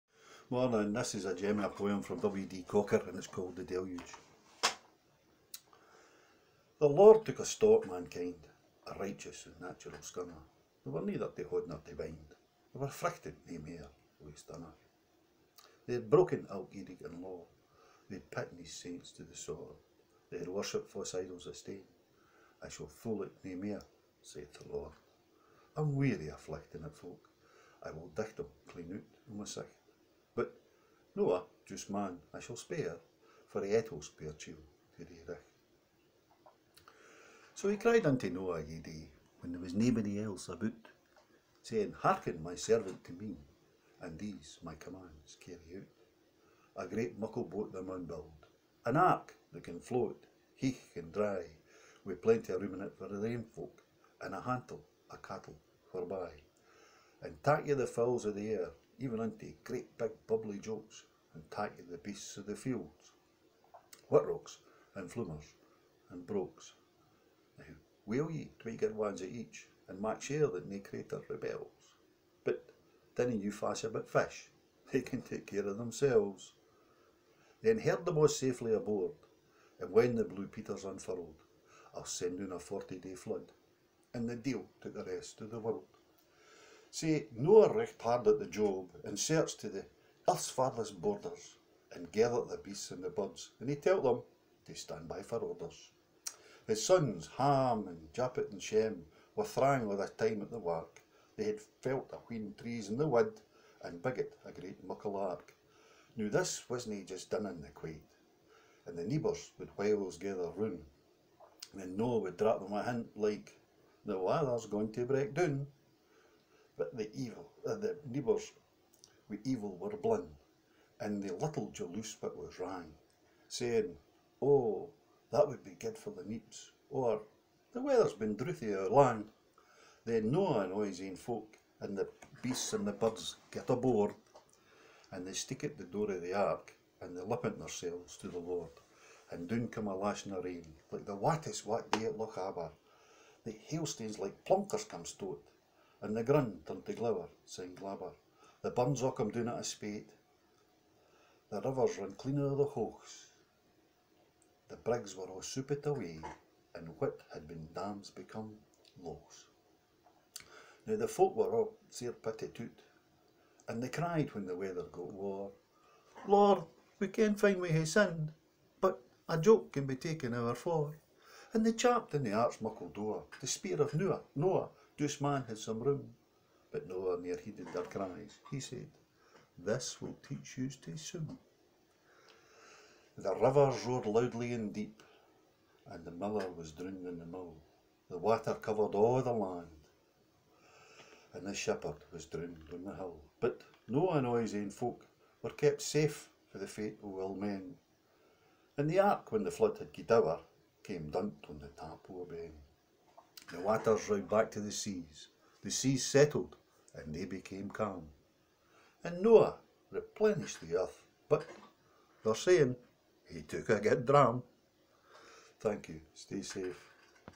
Jim Thomson - The Deluge
WD Cockers poem on Noah and the Ark